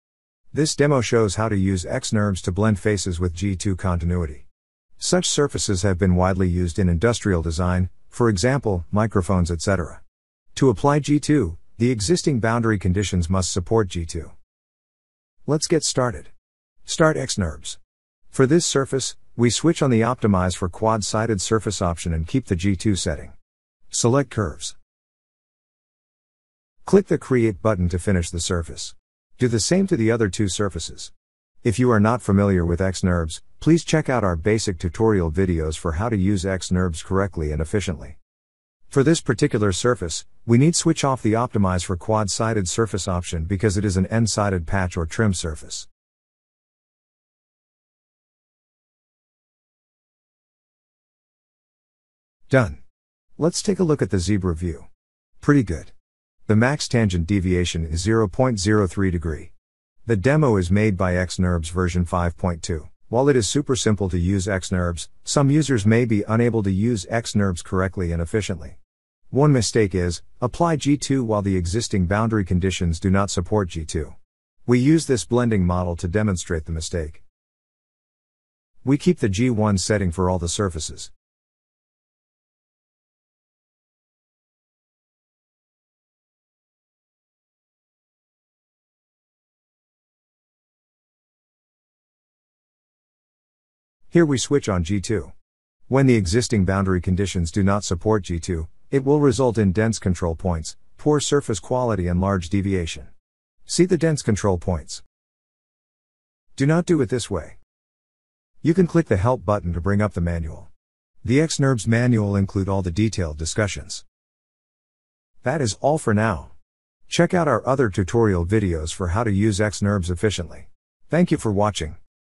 xNURBS Modeling: G2 Corner Blending
This demo shows how to use xNURBS to blend faces with G2 continuity. Such surfaces have been widely used in industrial design, e.g., microphones etc.. To apply G2, the existing boundary conditions must support G2.